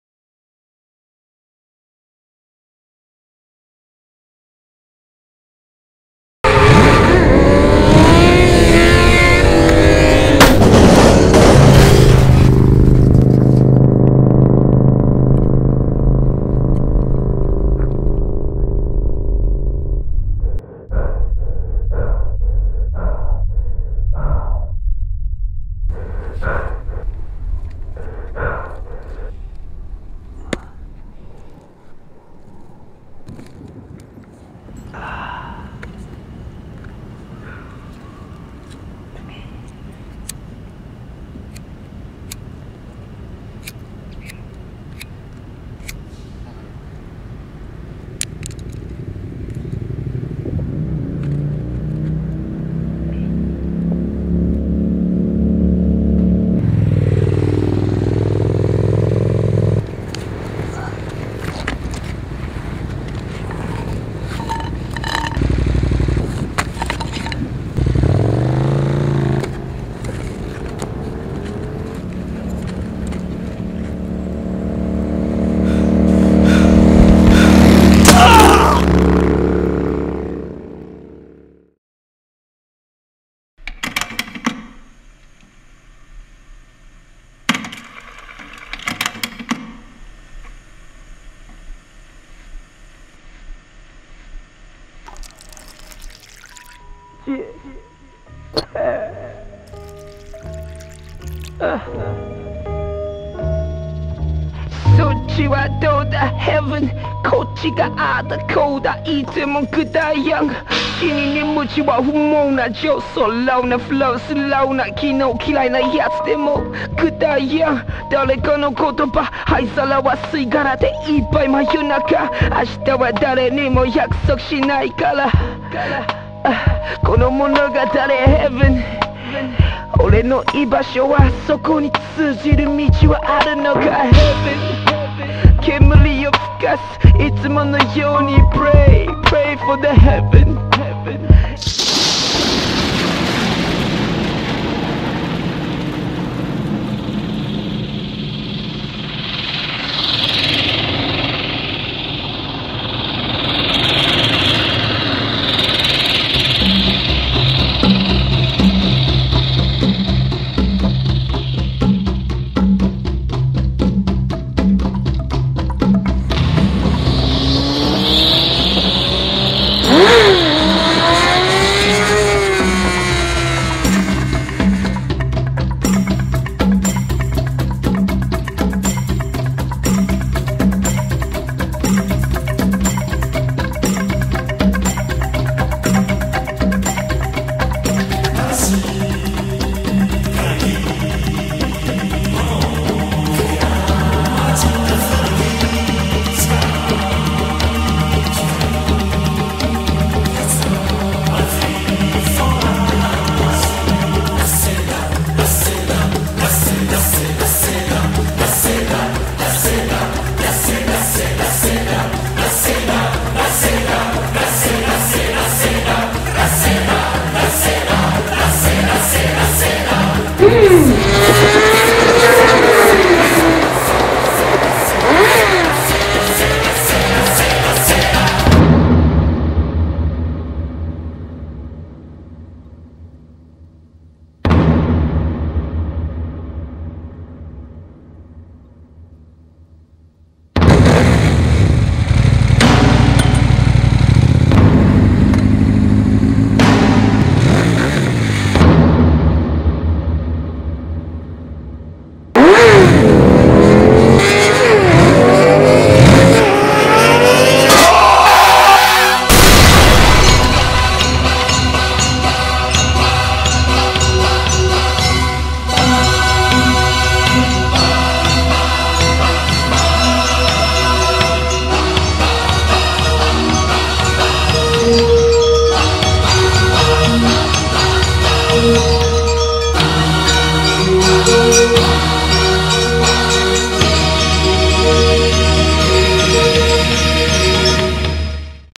Akira: The Kaneda Short Film (live action)
This film was inspired by Akira: the classic anime and manga.

Shot 90% guerrilla. Equipment used includes canon xh-a1 along with the letus extreme for select shots, and the canon 5d mark 2. Software used: Adobe After Effects, Premiere and Photoshop.

Thanks to everyone who helped me out, no matter how minor the help was. This would have been impossible to make without any of you.

[ January 28, 2010 ] Seeda (the rapper of the opening song) hears of this video all the way in Japan and approves.

Rough translation of blog: "[title] Some dope unofficial shit my friend put me up on

This is really high quality, so I'll pretend I didn't see this (e.g. I'm not gonna sue him). Holla."...